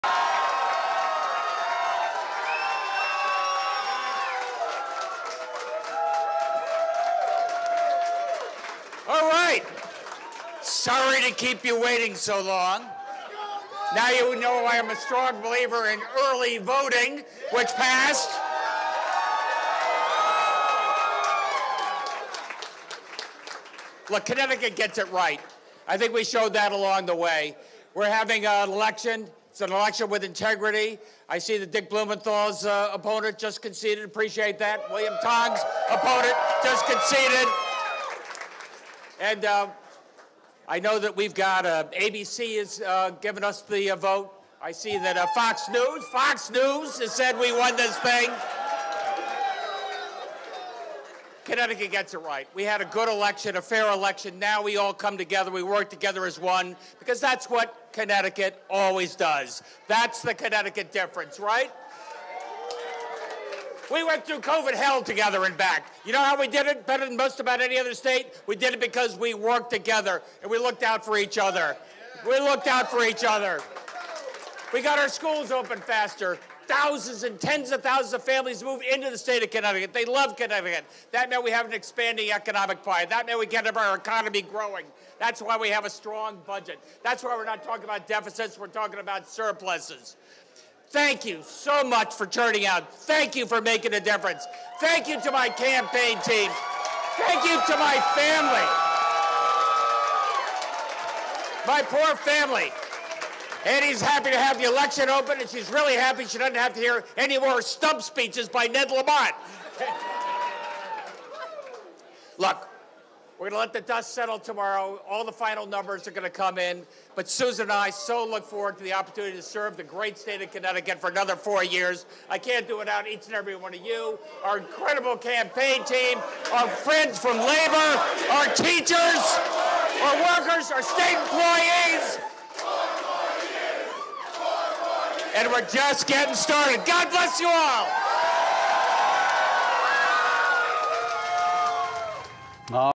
Gov. Ned Lamont speaks to supporters after Midterm Election
Lamont believes he's won the election but will wait till the final numbers come in.